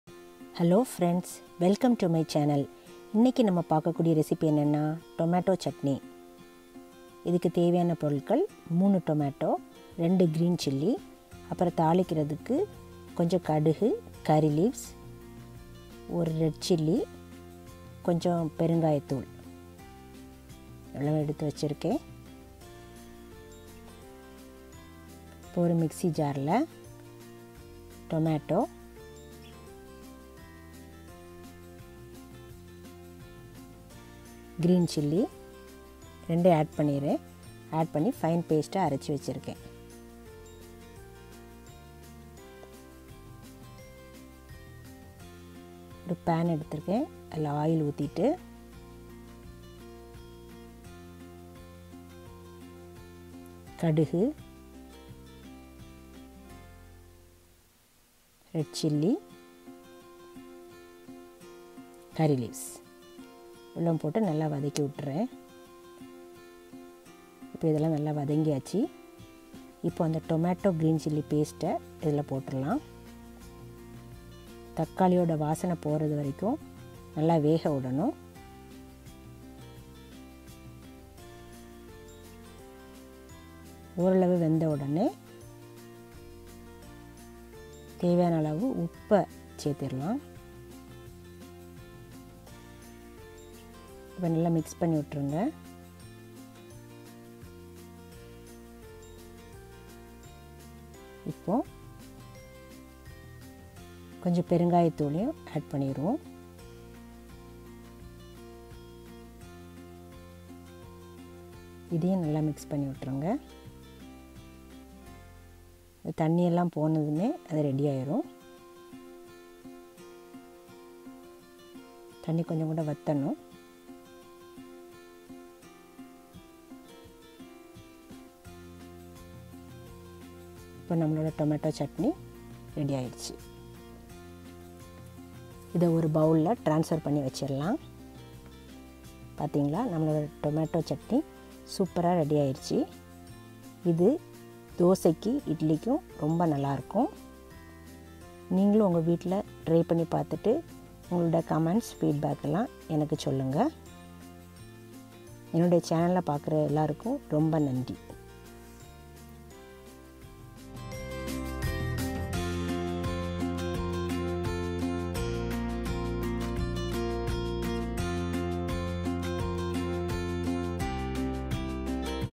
Tomato Chutney ( without onion)
Simple procedure to make Tomato Chutney. I hope you will try it and like it. Thank you 😊